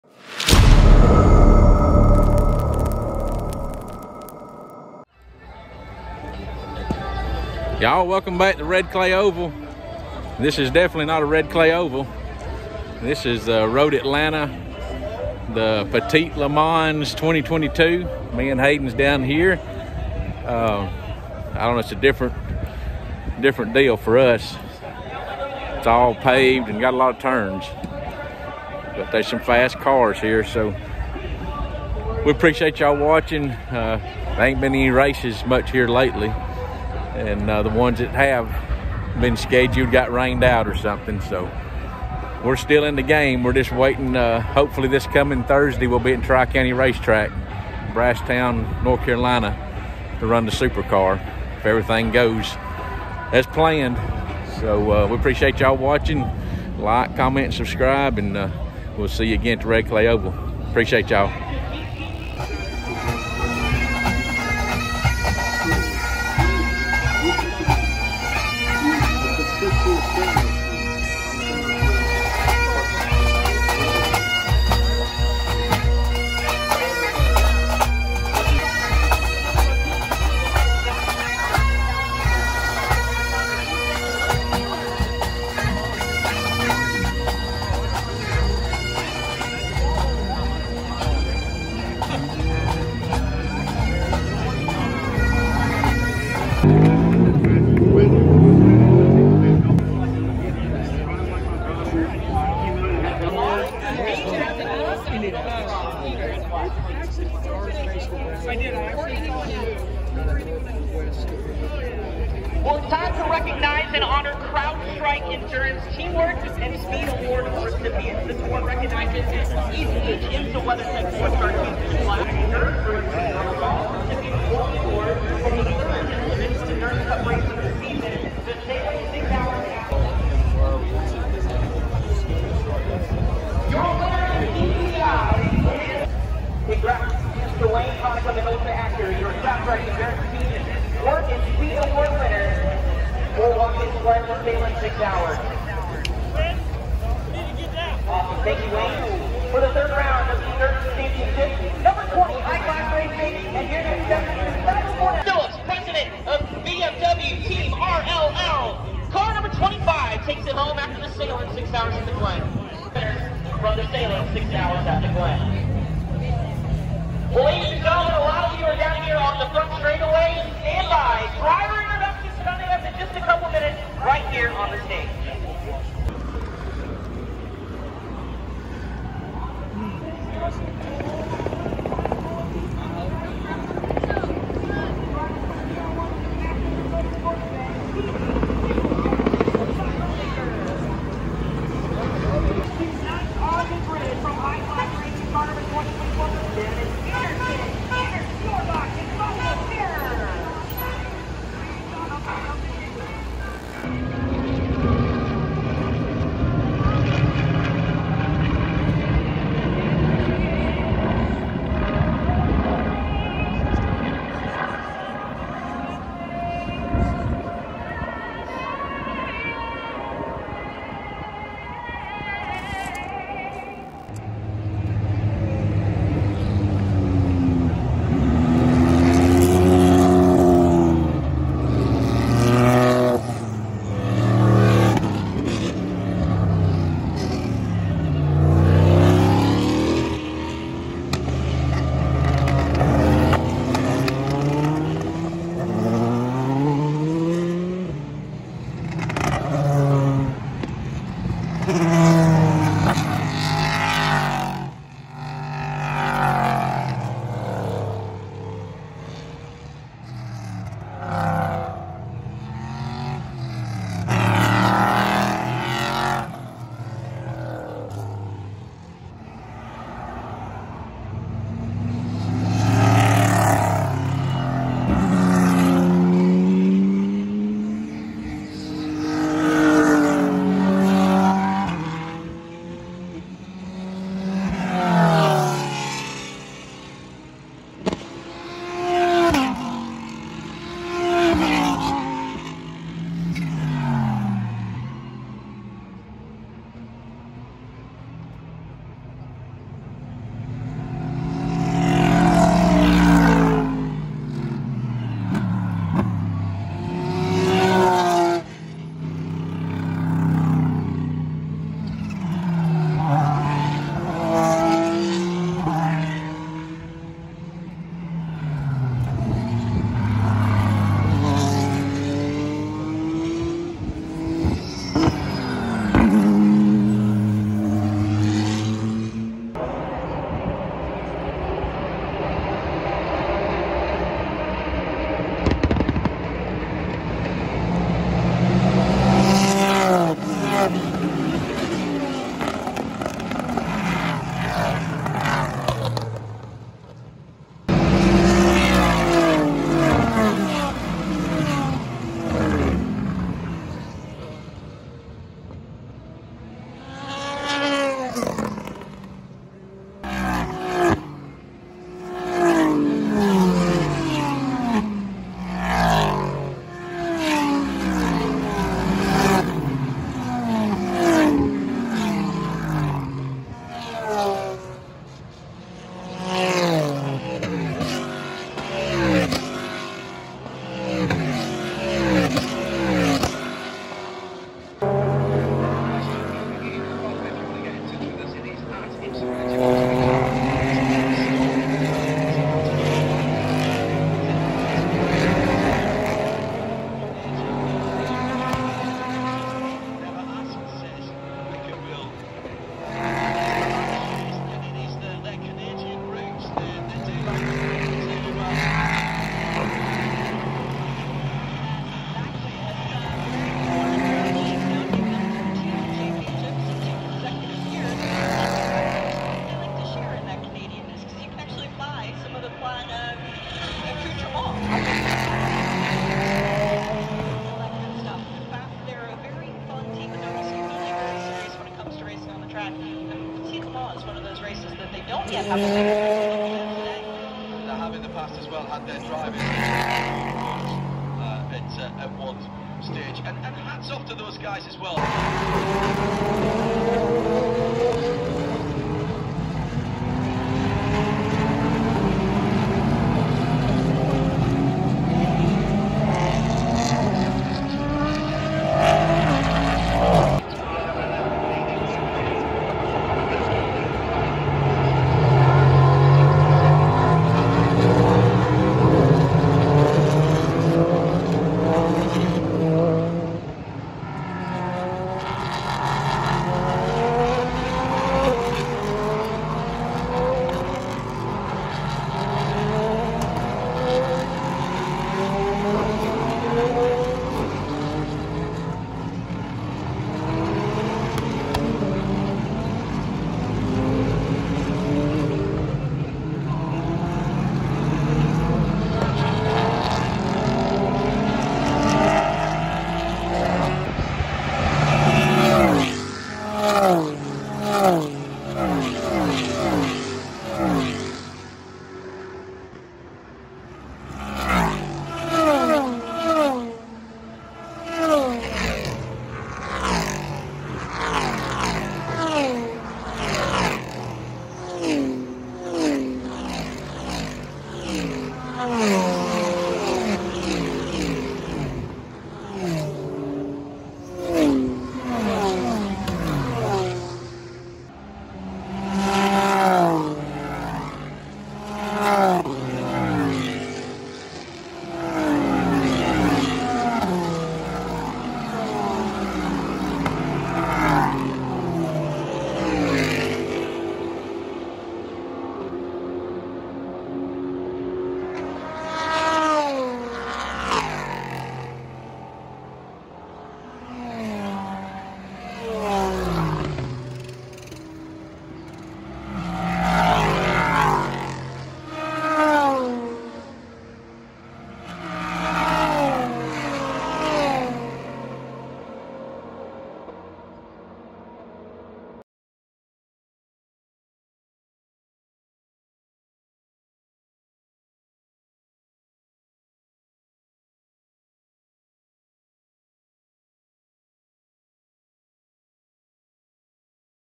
Petit Le Mans 2022 Highlights / Gridwalk / Racing
Petit Le Mans 2022 Highlights from Road Atlanta raceway in Braselton, Georgia. Part of the Red Clay Oval crew attended the 2022 Petit Le Mans at road Atlanta on Saturday October 1st, 2022. The petit Le Mans is a different style of racing than what we do, the highlights of the weekend is included in this video. We are looking forward to our next race and bringing you highlights for the rest of the 2022 race season. We will see you again at the Red Clay Oval.